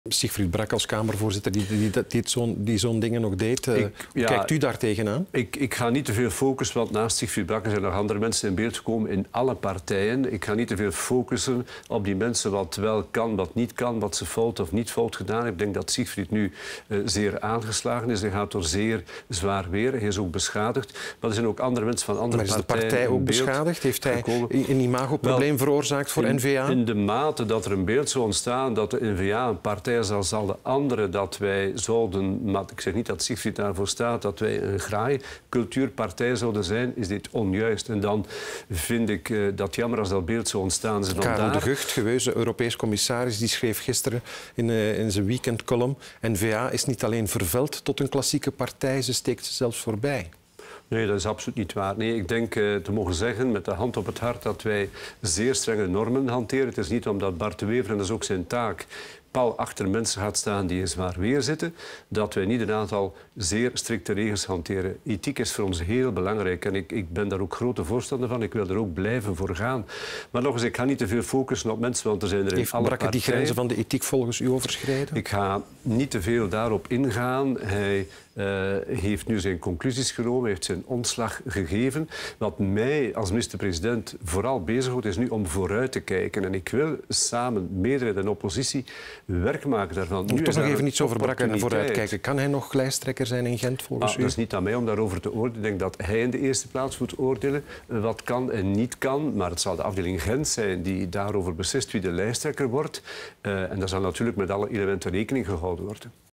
Postjespakker Geert Bourgeois (N-VA): "Siegfried Bracke is beschadigd"
copyright vtm